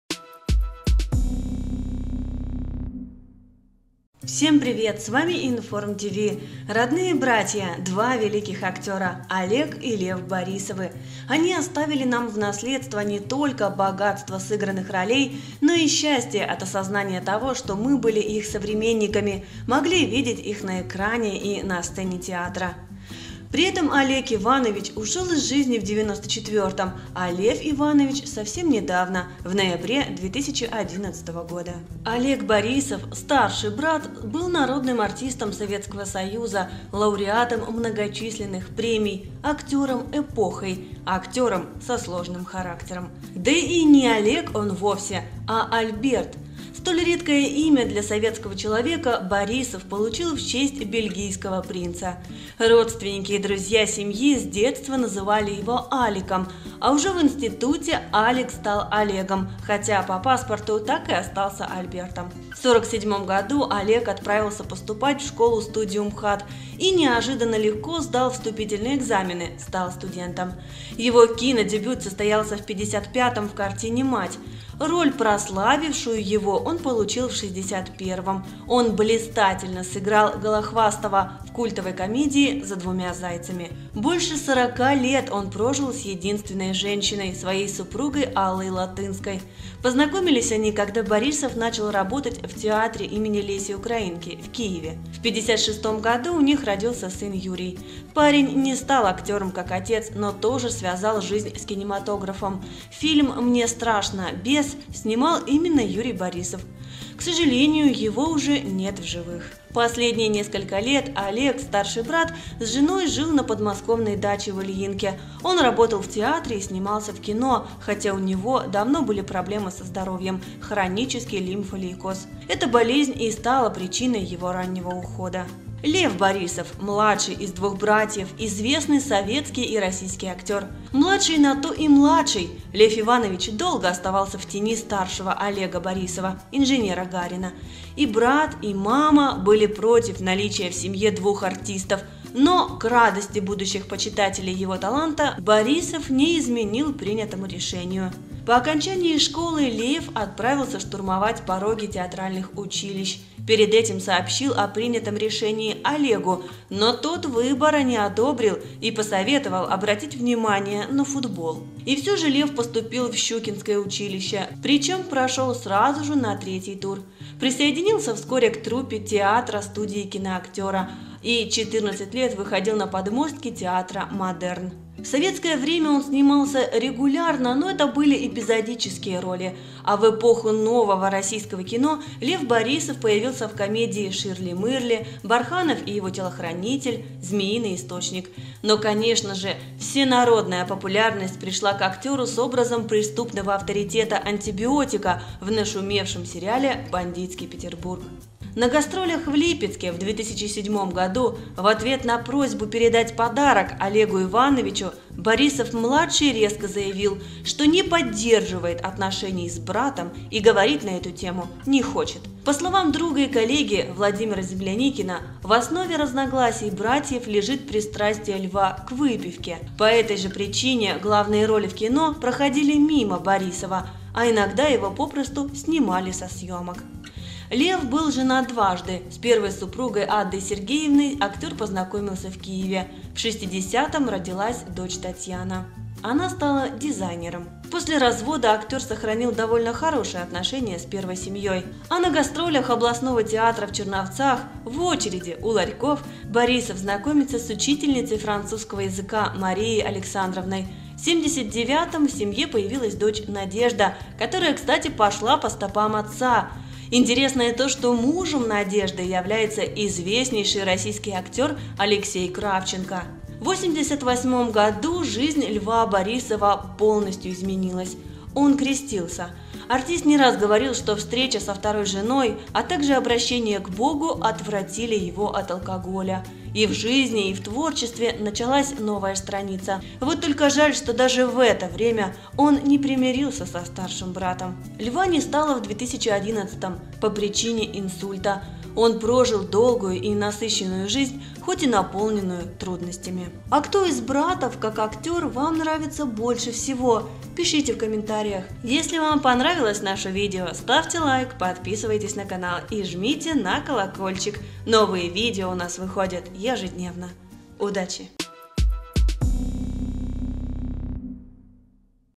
ЛЕВ И ОЛЕГ БОРИСОВЫ | РОДНЫЕ БРАТЬЯ, КОТОРЫЕ НЕНАВИДЕЛИ ДРУГ ДРУГА
Олег и Лев Борисовы были родными братьями. Но изнчально старший брат был против того, чтобы младший становился актером. Именно из-за этого их отношения испортились и они никогда не общались друг с другом.